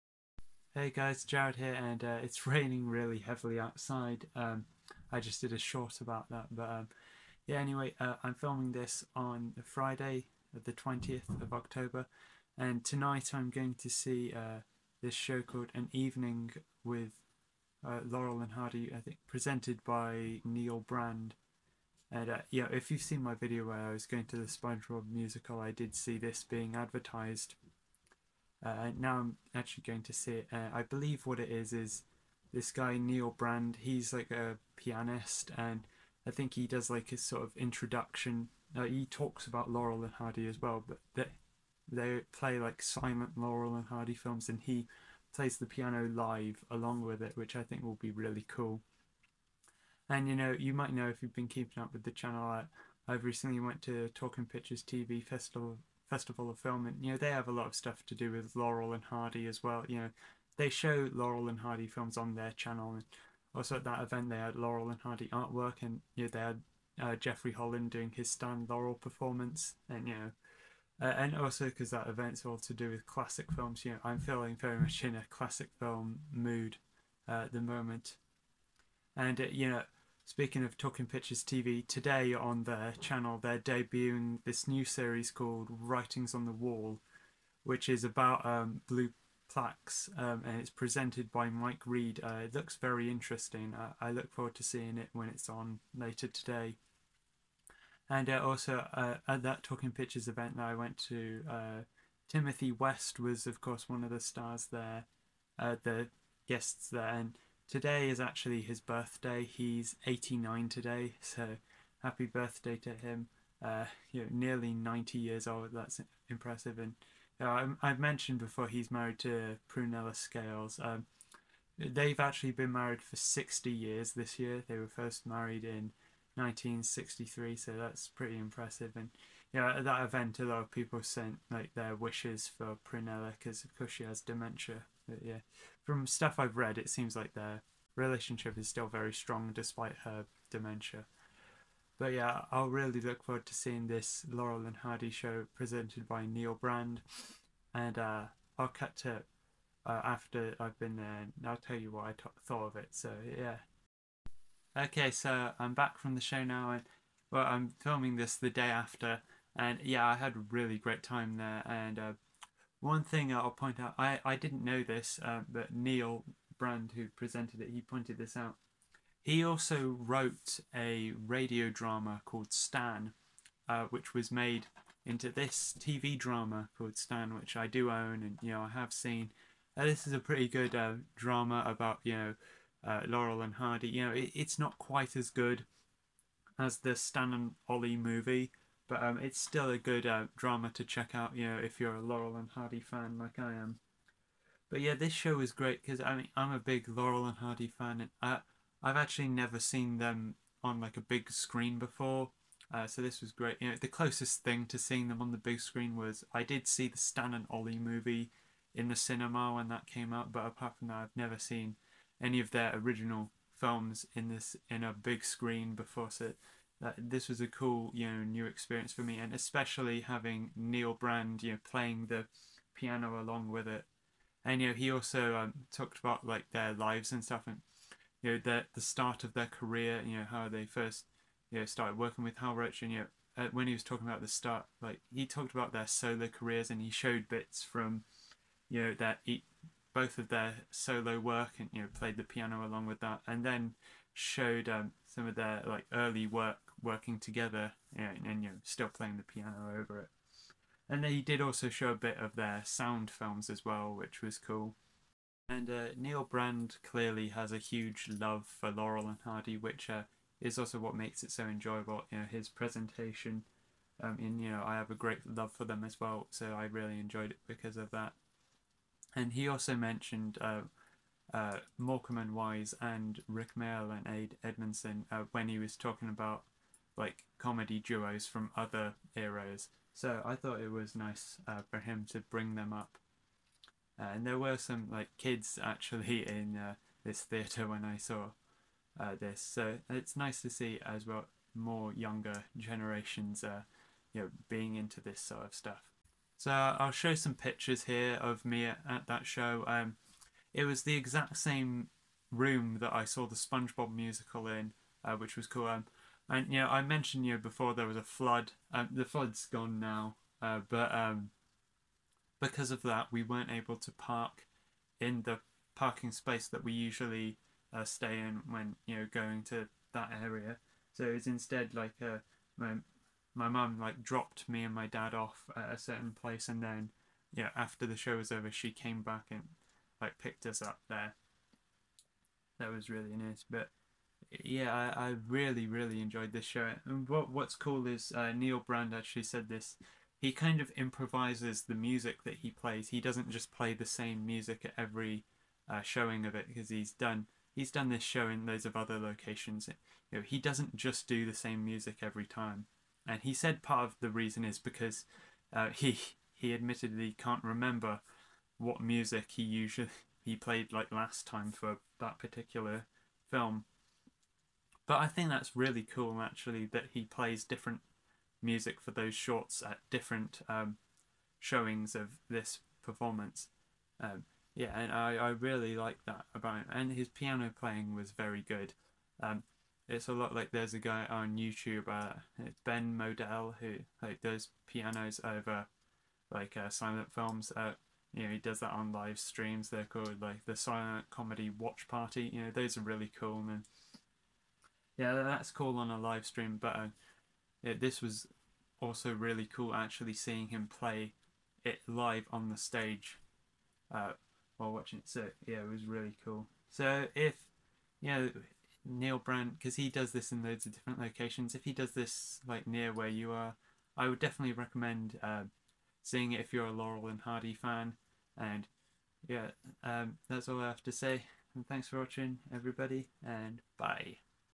Neil Brand's Laurel and Hardy show: my experience and review
I had a great time with this show, I hope you enjoy hearing me talk about it.